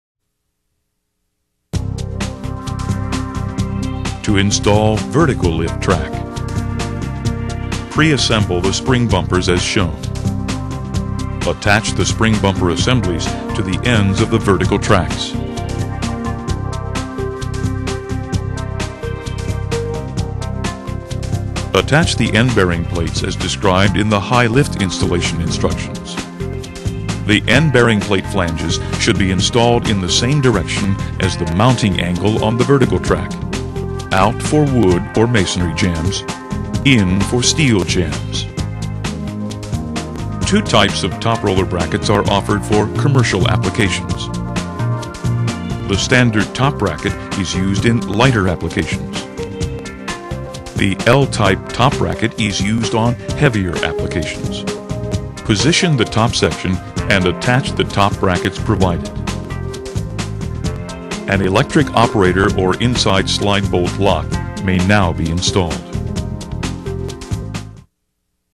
(6) Installing Vertical Lift Track
Clopay instructions for installing a commercial vertical lift track.